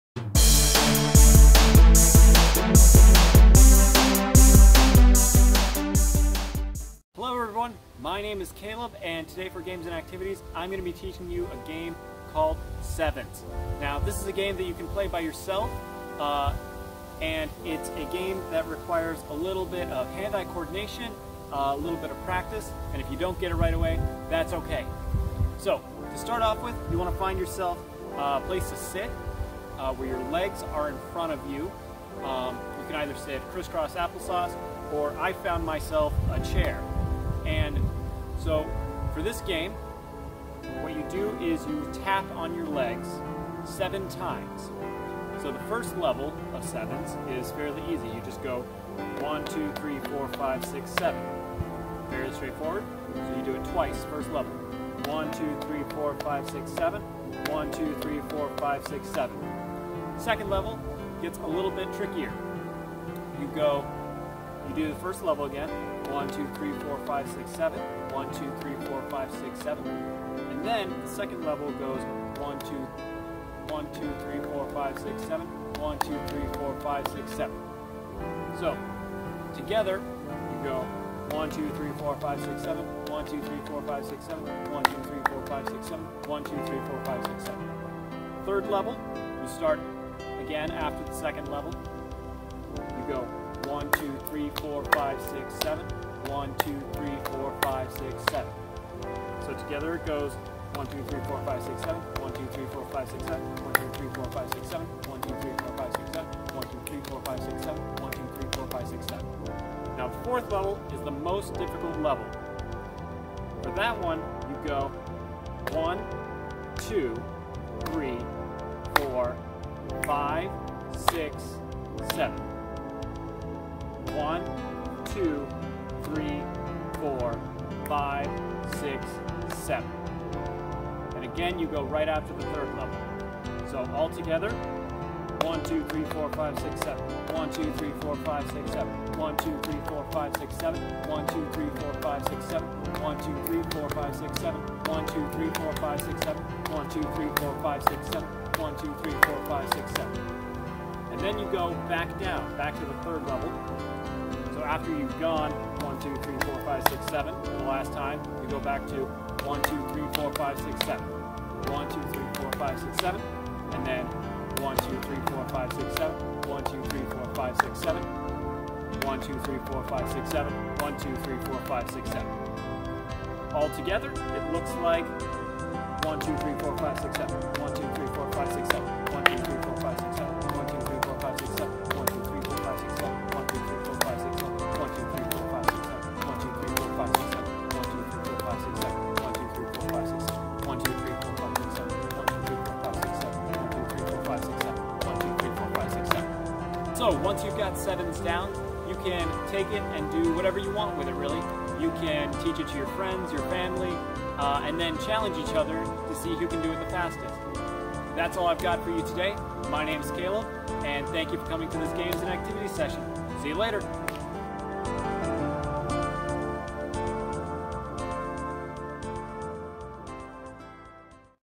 Sevens Game
This is a fun game to practice in your free time!  You can play this with many people or just by yourself!  Get your hands ready and enjoy!